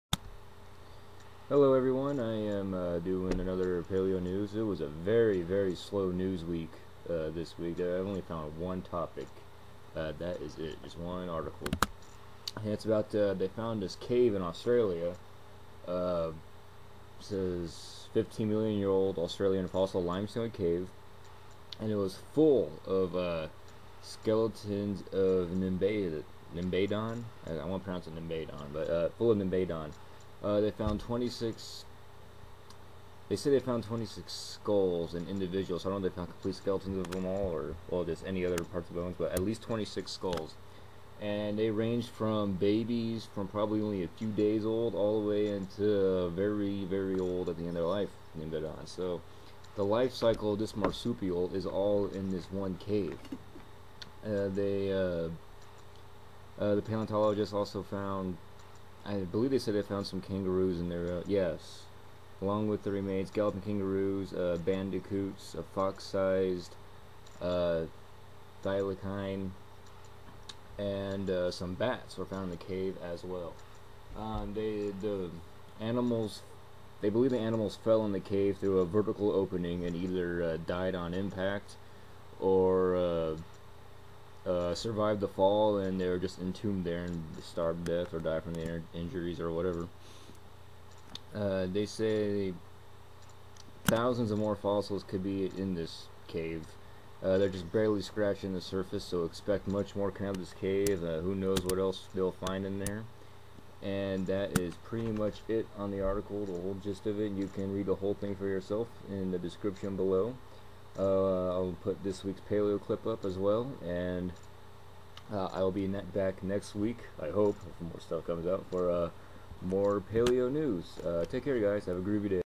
Fossil Cave
THIS WEEKS PALEO CLIP

A cave discovered in Australia housed many fossils and possibly thousands more